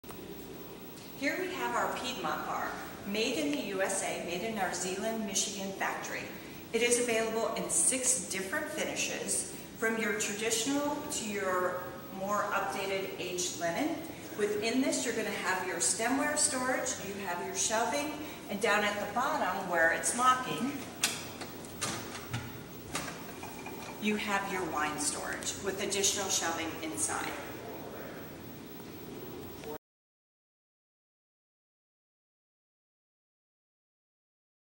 Howard Miller Piedmont V Corner Wine & Bar Cabinet 690046 at Home Bars USA
More information about Howard Miller Piedmont V Corner Wine & Bar Cabinet 690046

This traditionally styled Wine & Bar cabinet combines plentiful storage with a convenient space-saving corner design. Aged Linen finish on select hardwoods and veneers, wire brushed for a natural, reclaimed wood appearance. The lower cabinet features a metal wine rack with a capacity of 13 bottles, and two fixed shelves, which offer storage for spirits, mixers, and bartender's guides.

Locking door with glass panel for added security. Touch-Lite adjustable light switch (upper right hinge on lower door) offers four levels of lighting: low, medium, high, and off. Glass mirrored back panels in upper cabinet. Adjustable levelers under each corner provide stability on uneven and carpeted floors. Glass shelf can be adjusted within the upper cabinet. Pad-Lock cushioned metal shelf clips increase stability and safety.